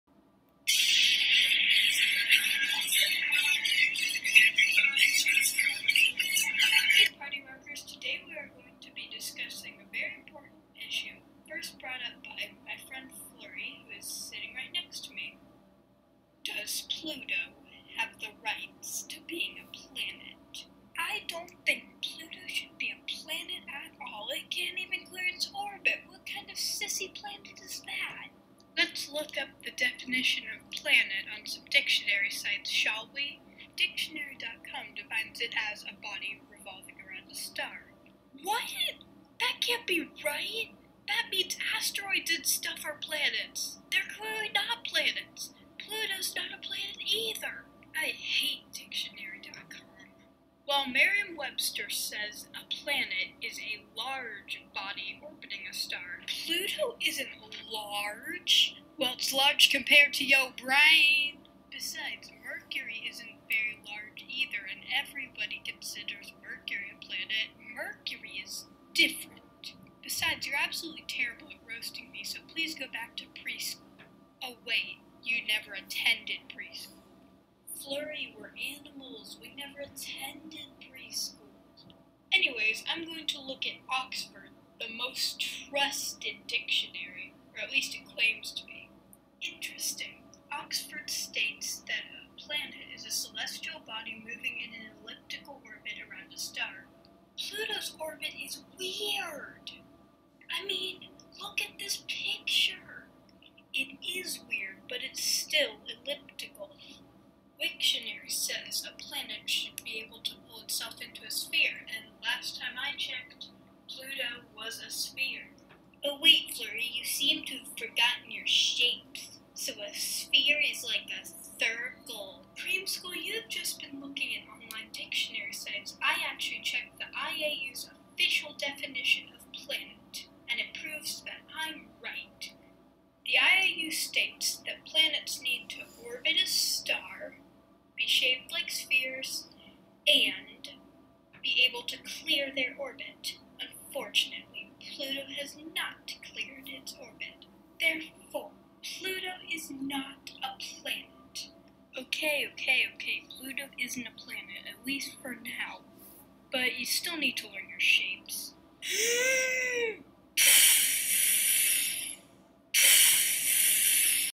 The Pluto Controversy
A dog and a bear argue over a scientific concept with the help of dictionary sites, Wikipedia, and even...roasts? Holy cow!
Yes, I now have a new intro.
CHARACTERS (both mine):
-Creamsicle (orange dog)
-Flurry (white bear)